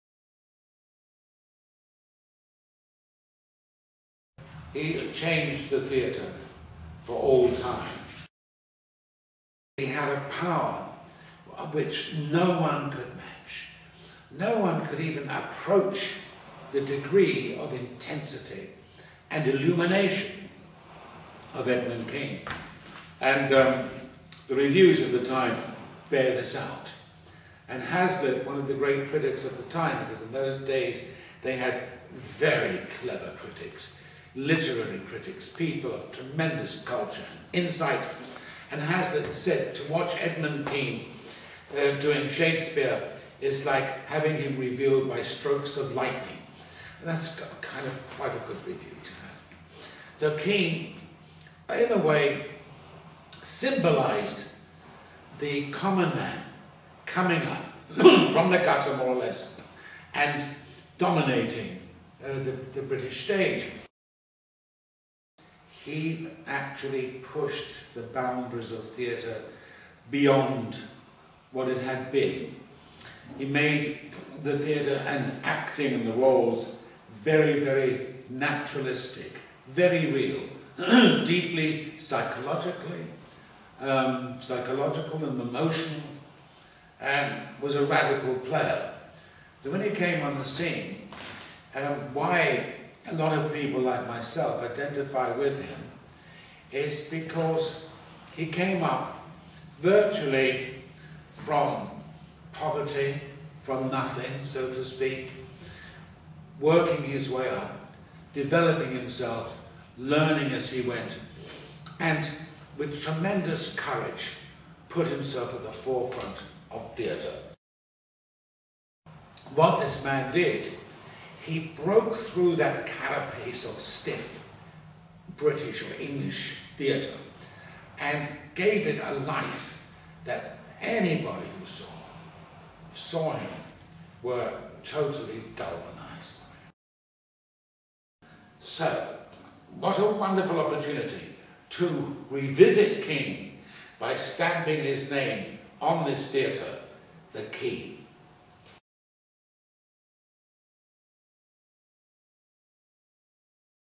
Steven Berkoff talks about Edmund Kean
Steven Berkoff speaks about Edmund Kean and why a theatre should bear his name.